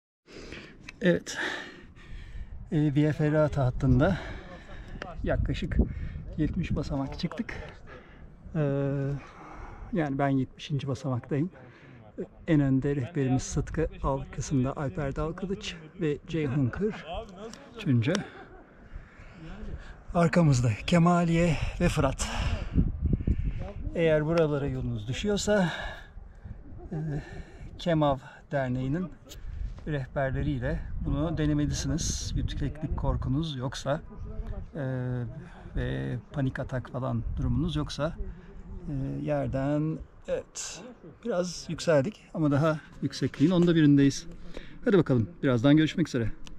Kemaliye Via Ferrata
Türkiye’nin ilk ve tek via ferrata hattının girişi. 517 basamağın 70.sinde kısa bir video çektim.
Demir basamakları çıkmak değişik bir his, korkunuz-paniğiniz yoksa çok da zor değil.  Vlog yakında.